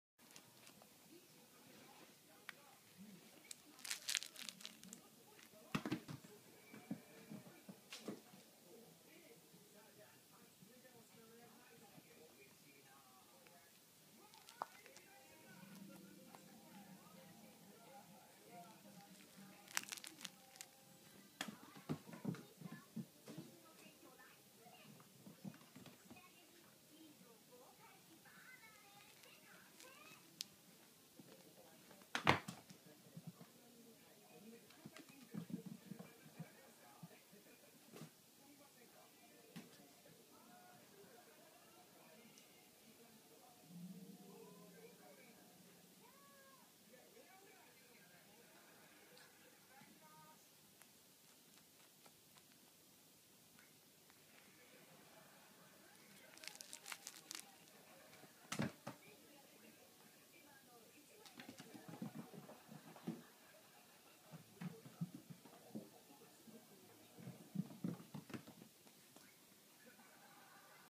一緒に遊びたい😍
とってこい遊びが上手な子猫。

📸 taa04 @kurotaa(Twitter)